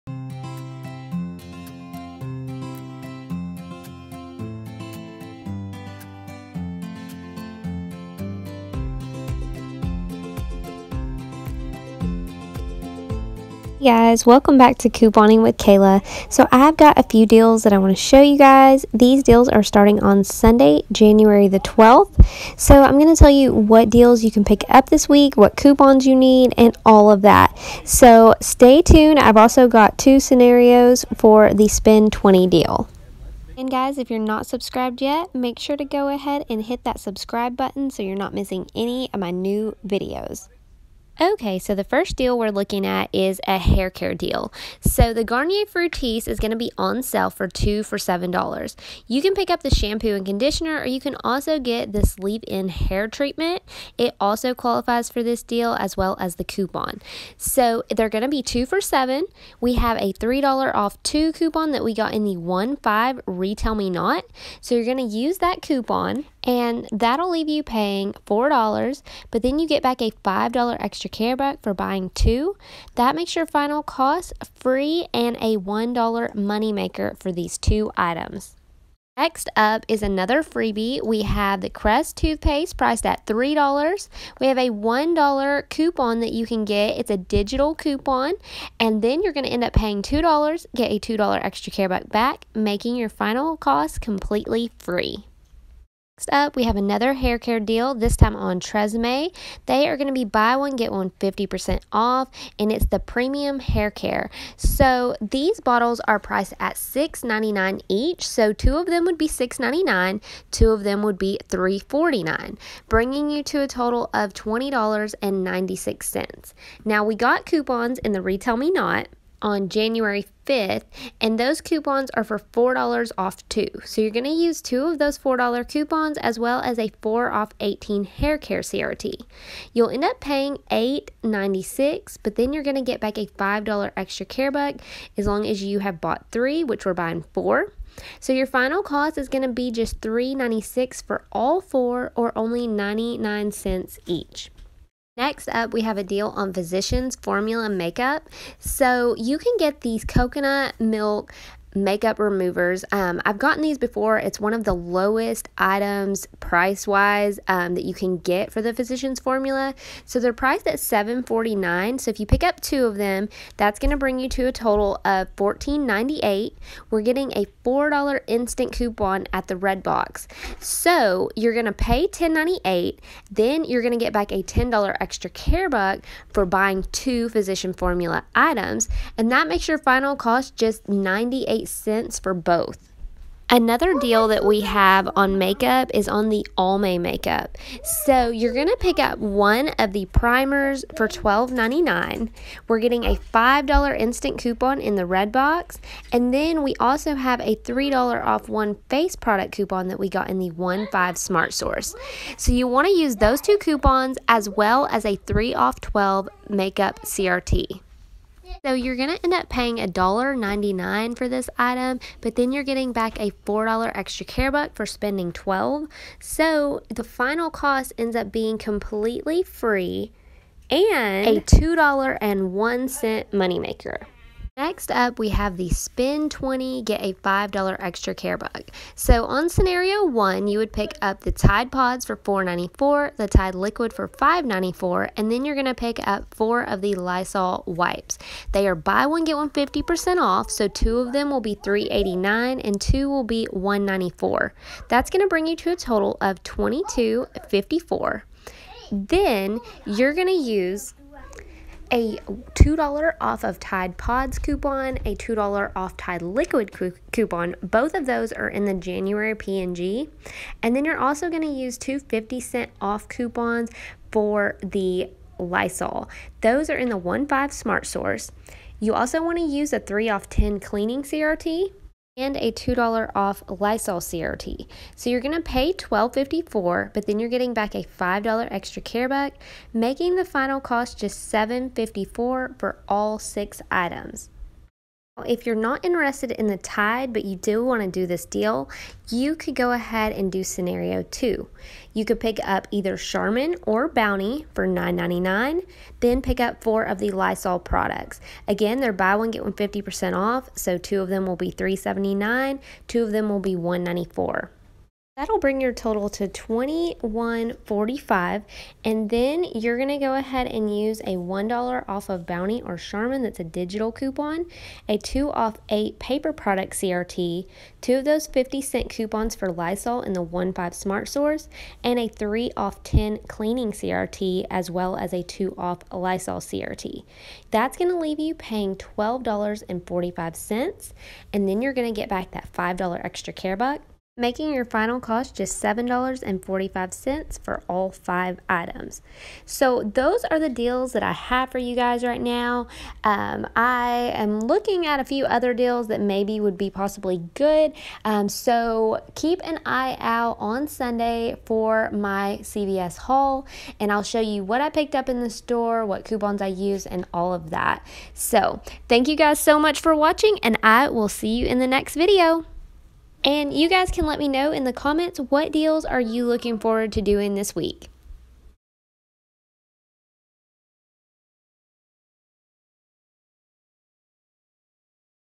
CVS Deals To Do 1/12-1/18 Freebies & MoneyMakers | Spend $20 Get $5 Scenarios
(You Get money for referrals on this one and earn cash when you use your card at Dunkin Donuts, Papa Johns, & others!)